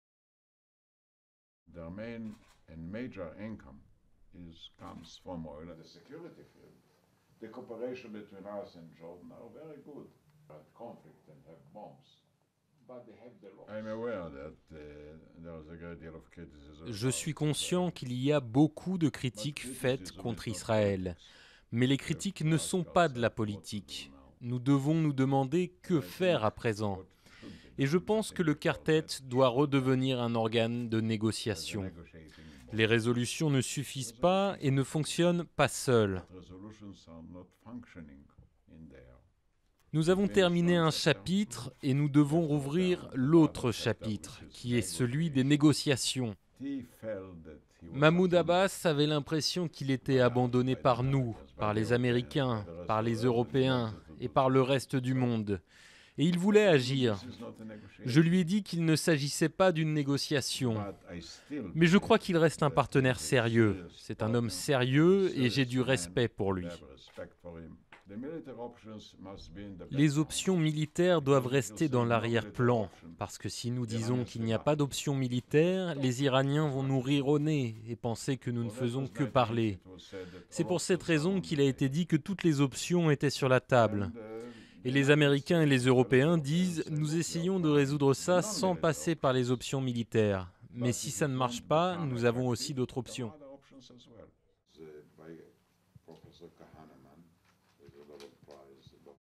Peres: Abbas reste "un partenaire sérieux" pour la paix
Le dirigeant palestinien Mahmoud Abbas reste "un partenaire sérieux" pour la paix en dépit de sa démarche à l'ONU, où la Palestine a obtenu le statut d'Etat observateur au grand dam d'Israël, a déclaré le président israélien Shimon Peres dans une interview exclusive à l'AFP. Durée: 01:39